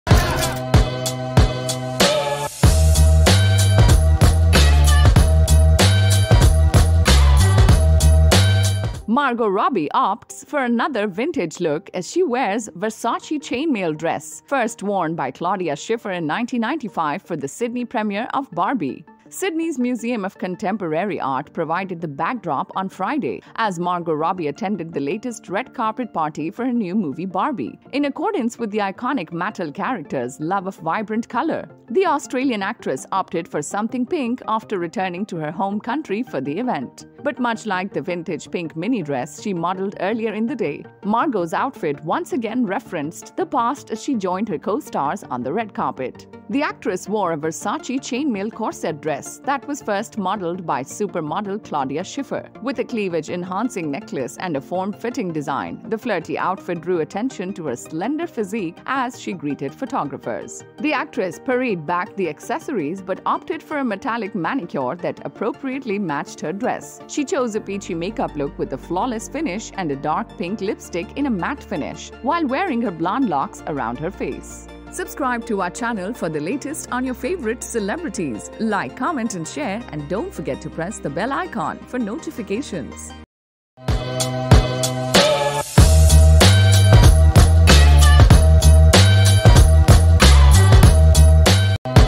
Margot Robbie Channels Vintage Glamour in Versace Chainmail Dress
Sydney's Museum of Contemporary Art provided the backdrop on Friday as Margot Robbie attended the latest red carpet party for her new movie Barbie.